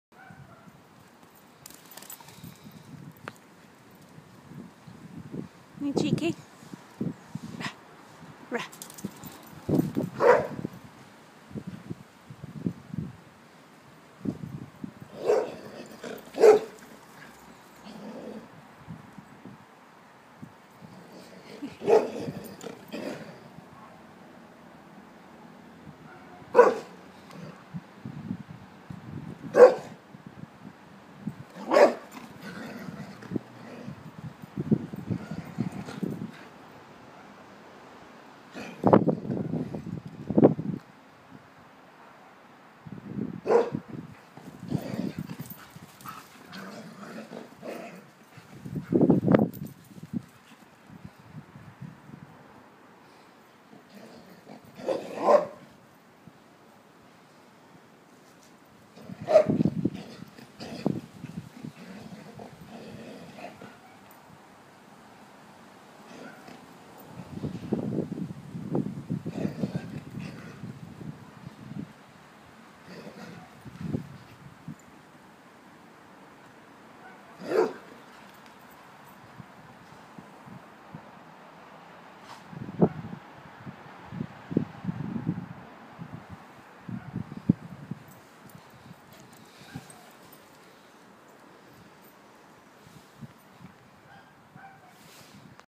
Megan and Rosie playing
Megan (Saluki/Greyhoud cross) & Rosie (Whippet) doing zoomies round the garden